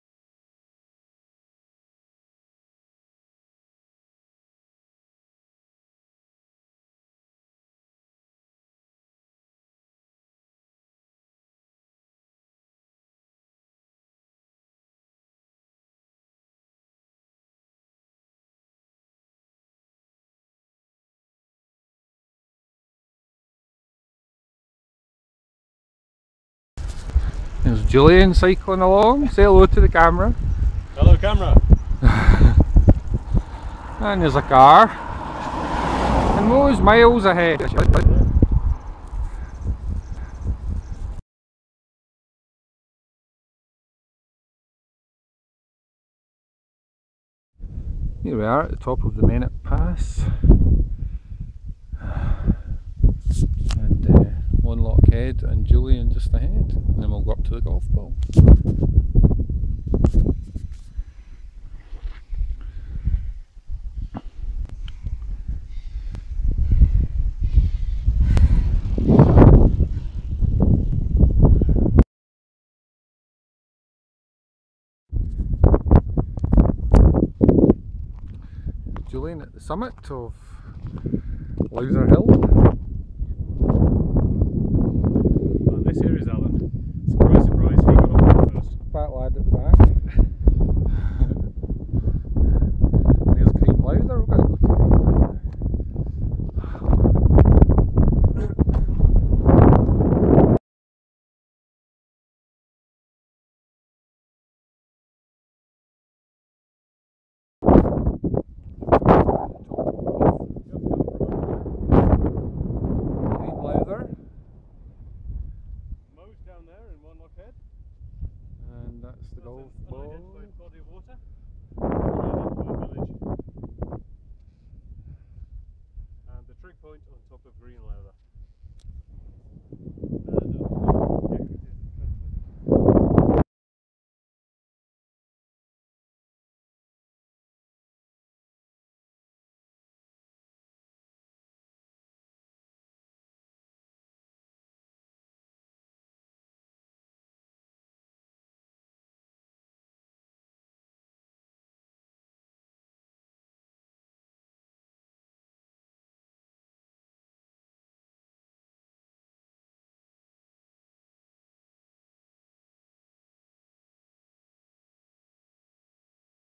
Green Lowther April19 2014
Ride to Green Lowther Summit (722m) Sanquhar - Mennock Pass - Wanlockhead - Green Lowther - Crawfordjohn.